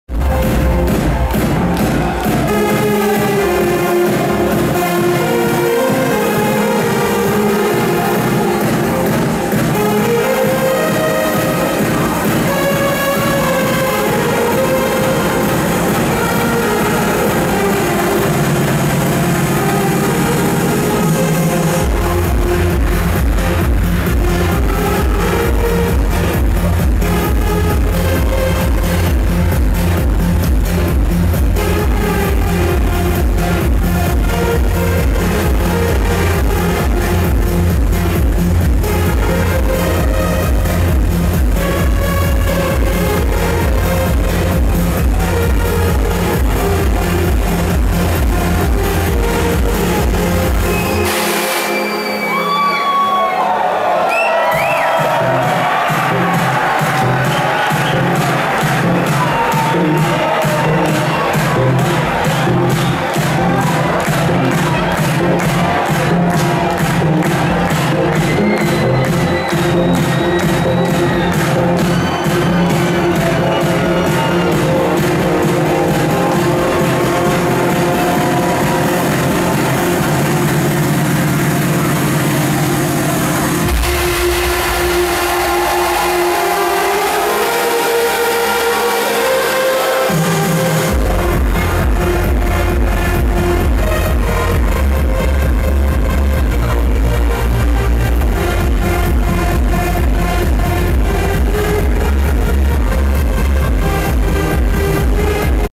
InStereo feat. Markus Schulz - Apr. 20th, 2013 - Video #11
InStereo feat. Markus Schulz - N. American SCREAM Tour
Stereo Live, Houston, TX, USA on Saturday, 20-April

Personal Video #11 - Sony DSC-HX20V